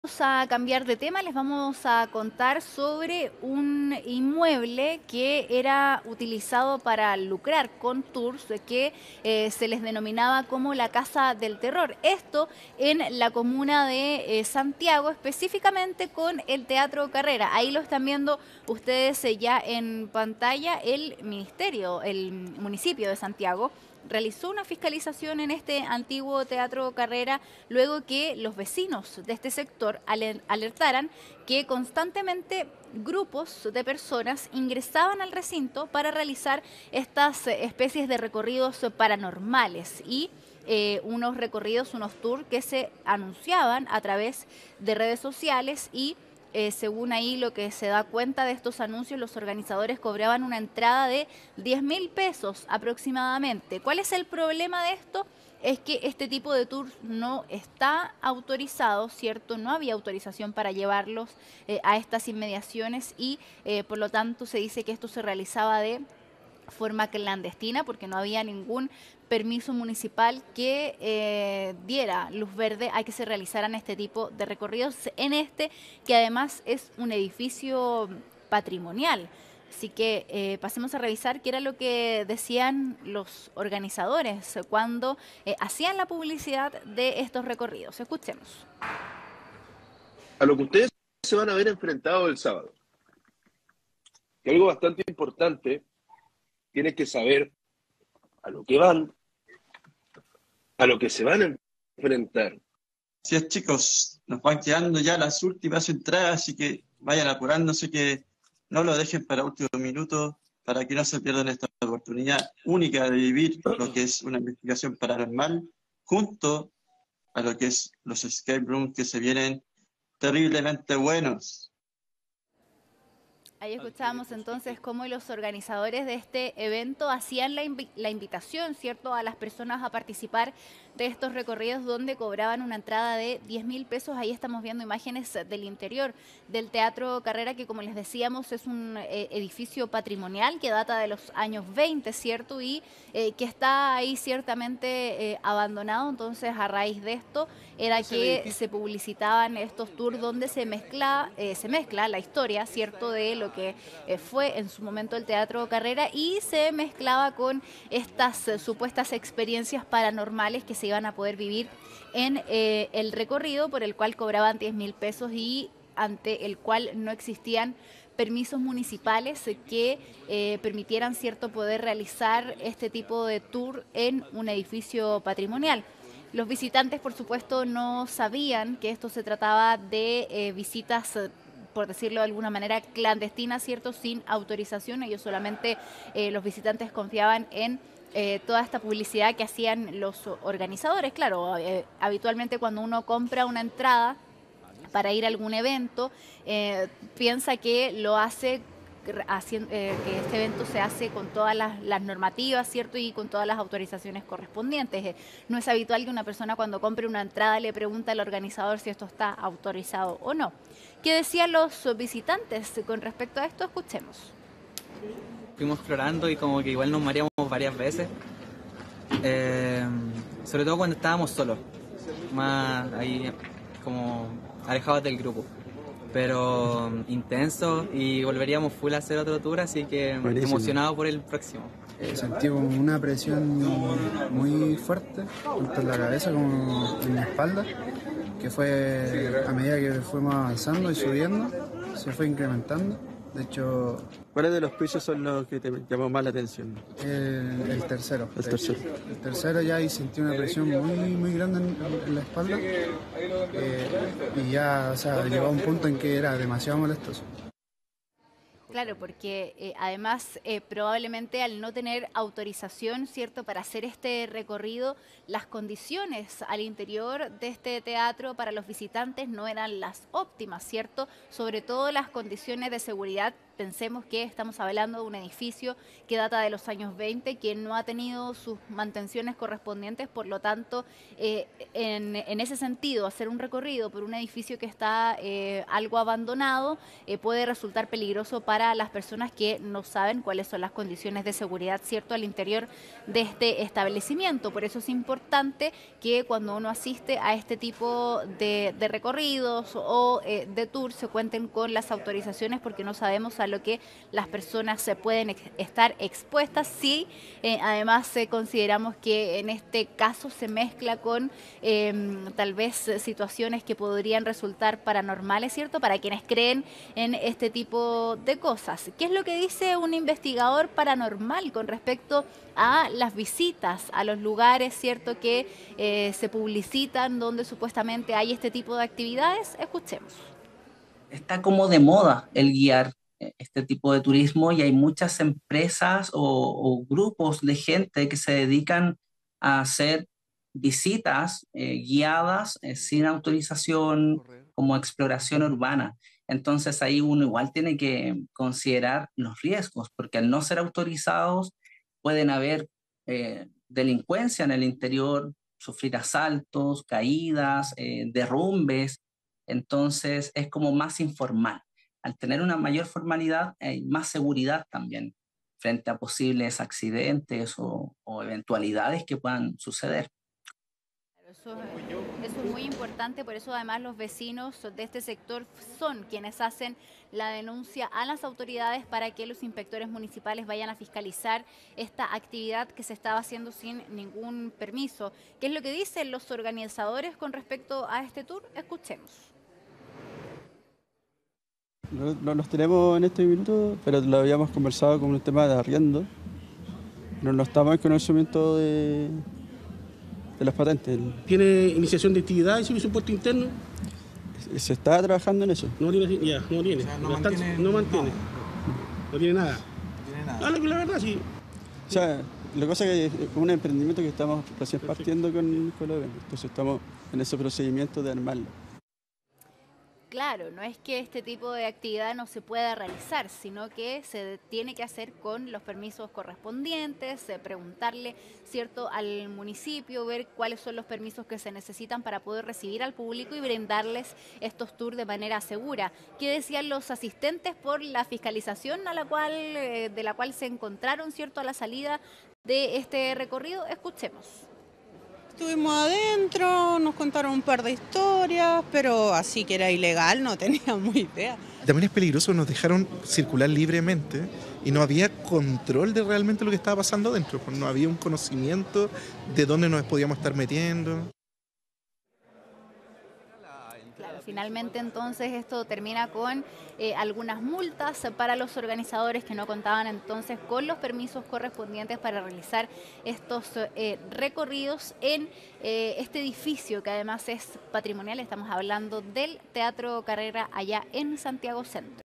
CLAUSURAN TOUR "PATRIMONIAL" en ex Teatro Carrera: Funcionaba de forma clandestina - CHV Noticias
Los organizadores de estos recorridos no contaban con ningún tipo de patente o permiso municipal. Tampoco mantenía las condiciones mínimas para garantizar la seguridad de los clientes. Además, cobraban $10 mil pesos por el ingreso a este patrimonio cultural.